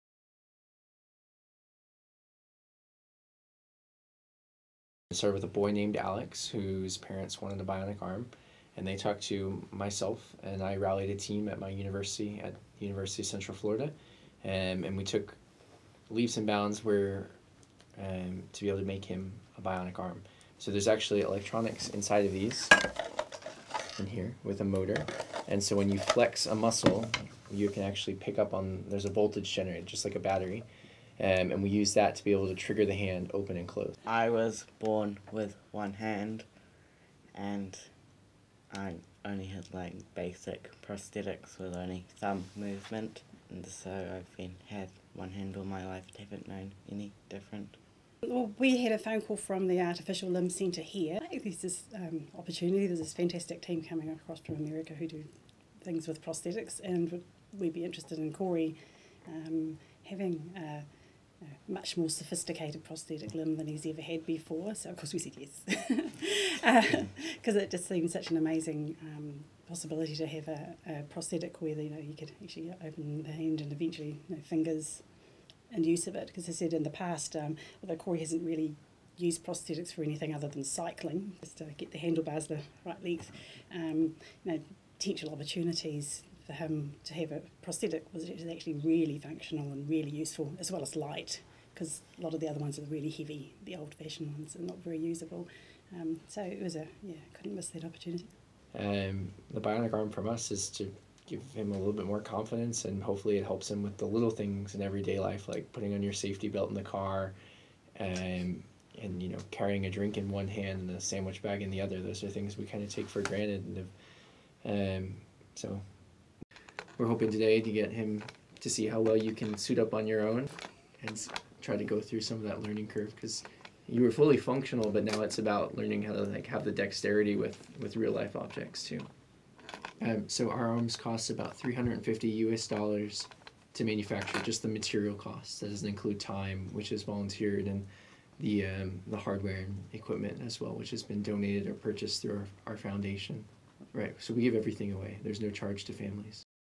Dunedin boy gets bionic arm
A Dunedin boy has a bionic arm for the first time